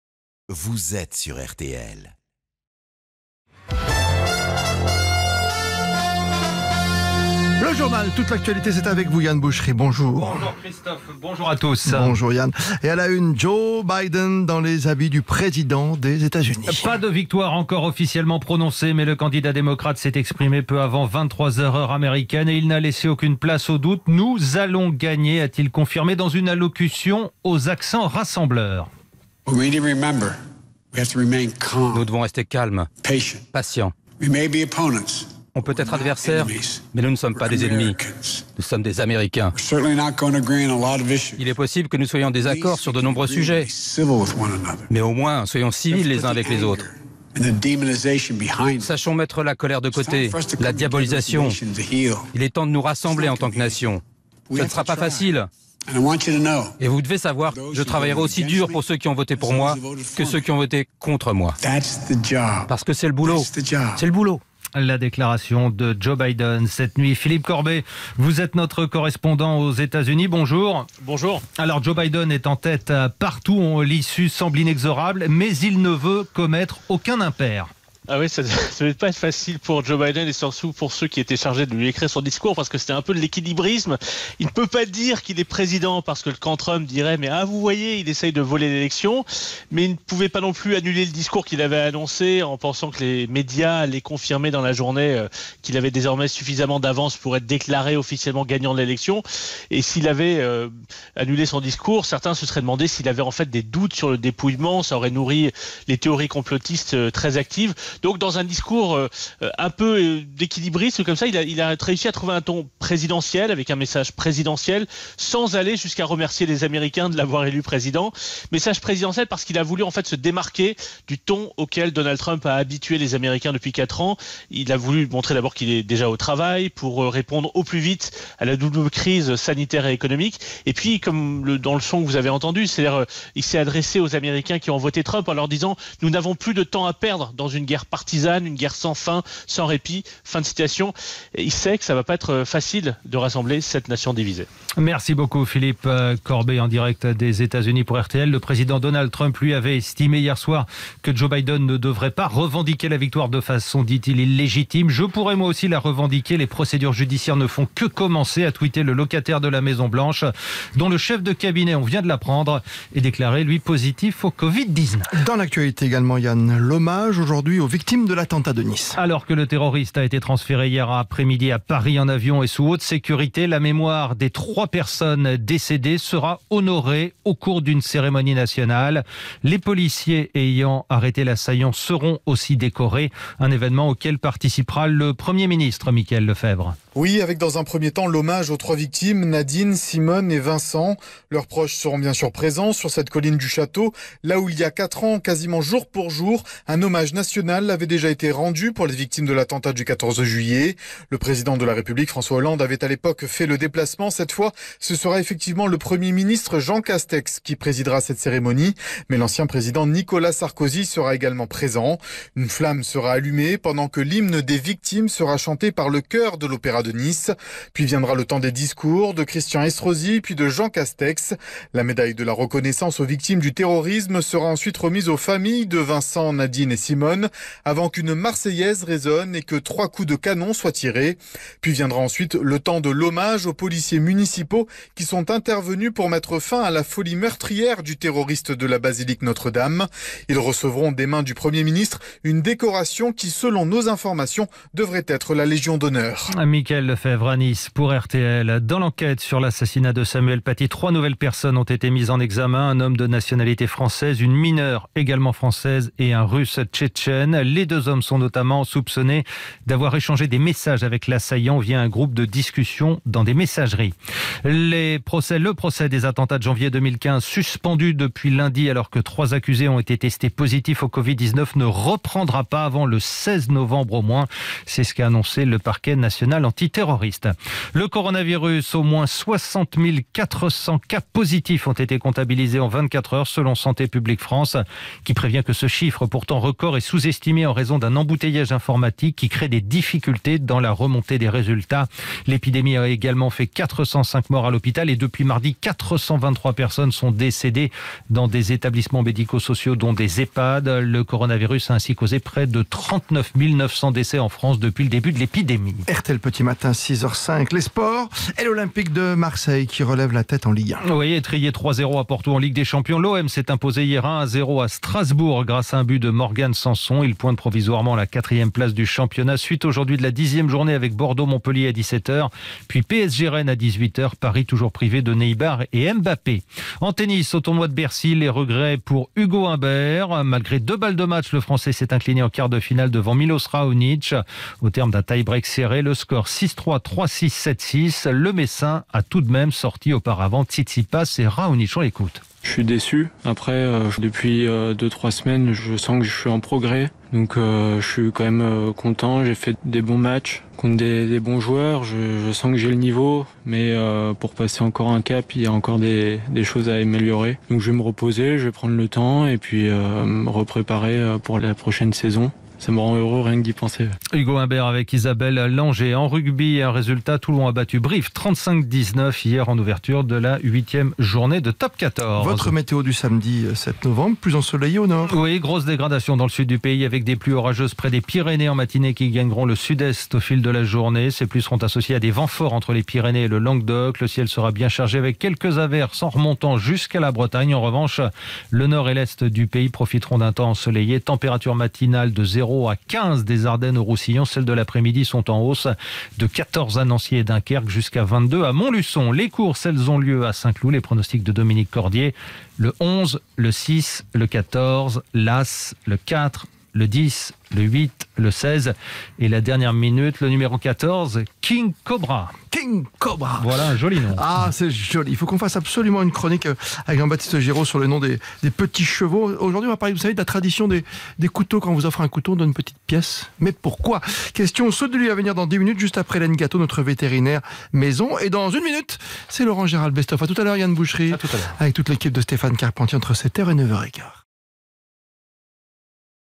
Les infos de 6h - Présidentielle américaine : Joe Biden, prudent malgré son avance
Joe Biden, le candidat démocrate, largement favori à l'élection présidentielle, a dévoilé les contours de sa future présidence ce vendredi 6 novembre au soir en direct de Wilmington dans un discours.